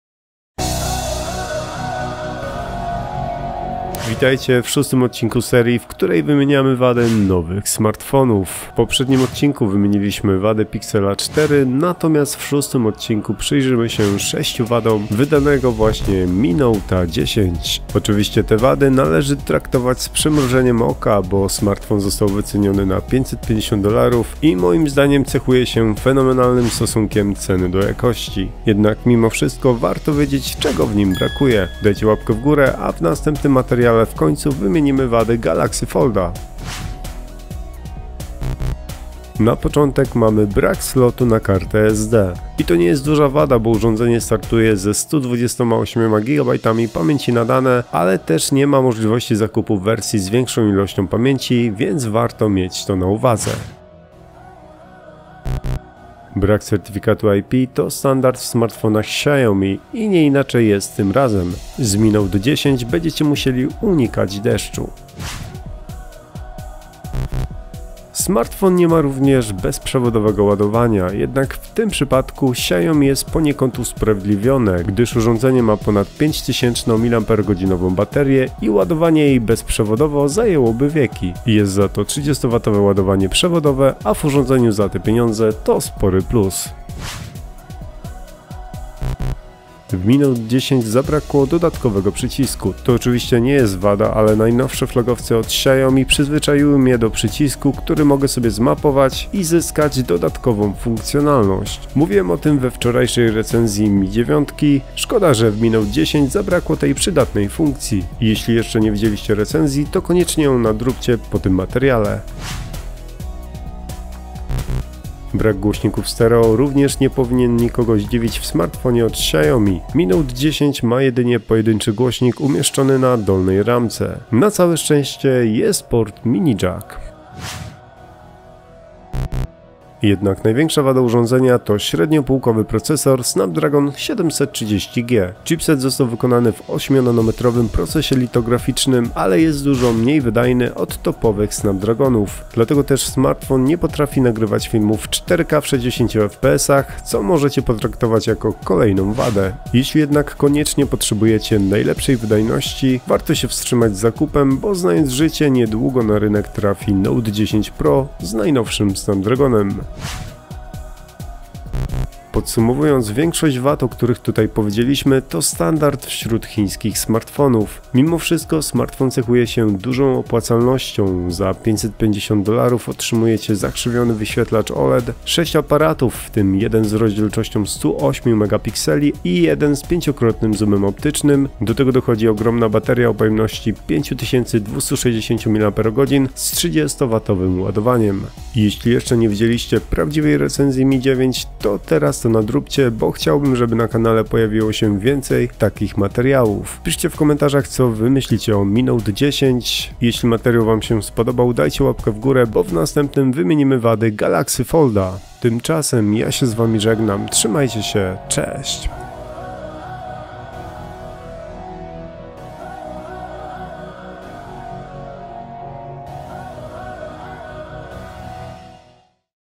Xiaomi Mi Note 10 ( CC9 Pro )⛔ Wady i braki Opinia pl | Premiera cena i specyfikacja
✅W tym materiale przedstawię 6 braków Xiaomi Mi Note 10 ( CC9 Pro )

✅Swoją premierę miał właśnie Xiaomi Mi Note 10, który został wyceniony na 550 dolarów. Smartfon cechuje się świetną specyfikacją. Największe wrażenie robią aparaty, wśród nich jest 108 mp matryca od Samsunga i 5x zoom optyczny. Smartfon ma również ogromną baterię o pojemności 5260 mAh, którą naładujemy 30W ładowarką. Czy jest to najlepszy smartfon do 2500 zł?